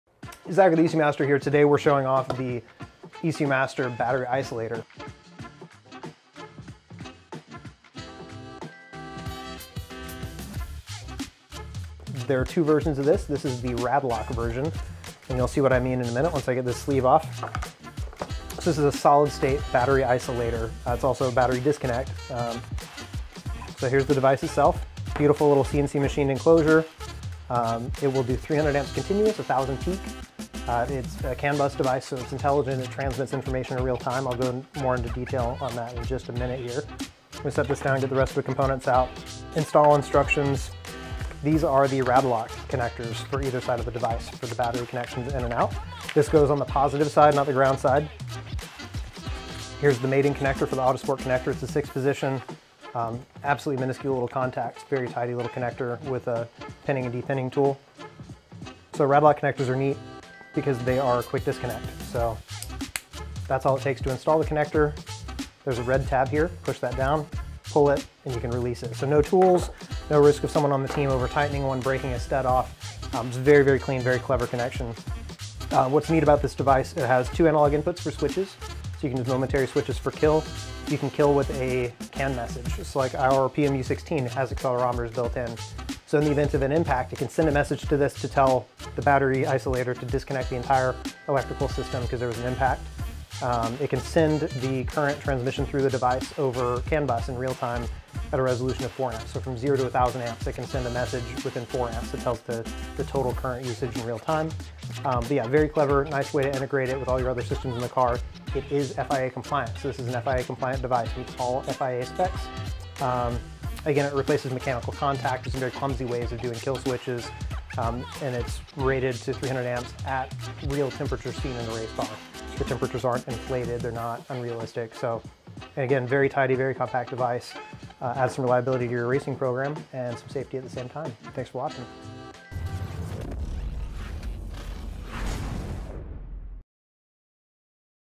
BATTERY ISOLATOR - RADLOK CONNECTORS UNBOXING
BATTERY ISOLATOR
Ecumaster Battery Isolator is a motorsport master relay device
designed for FIA compliant battery isolation and engine shutdown.
The device is designed for harsh motorsport environments and weighs
only 83g. Featuring a full solid-state design, an absence of mechanical
components ensures long life and high reliability.
The device has built-in alternator load dump protection without the
addition of any external components.
There is no risk of damage to expensive electronic equipment during
emergency shutdown. The isolator is controlled with two external
switches and can be shut down with a CAN-bus message.
The message may be sent from an Ecumaster PMU16 unit in the case
of an impact or another event. The device is configurable through CANbus.
It also sends diagnostic information which can be used by other
equipment and is valuable during device setup and diagnosis. It is fully
protected against overheating and over-current.
Device status is indicated with a multi-color led light.

 Autosport mating connector and terminals are included!

KEY FEATURES
•Weight: 83g
•Dimensions: 50mm x 100mm x 38.5mm
•Voltage range: 6V – 19V, 28V transient
(12V automotive installations only)
•Operating temperature range: -40C + 125C
•Current capability: 300A continuous, peak up to 1000A
•Current measurement resolution 4A
•Current measurement range: -1000A to 1000A
•Current measurement accuracy: ±10%
•Inductive load switching capability: 600mJ
•Enclosure: Anodized aluminium, IP67 rated
• High current terminals: M8 nickel plated terminals
or Radlok connectors (Radlock connectors included)
•Signal connector: Deutsch ASX202-06PN
(mating plug ASX602-06SN included)
•Power save state current consumption: less than 1mA
•ON state current consumption: ~20mA
•Engine kill output: 3A, High (VBat), Low (GND) or Hi-Z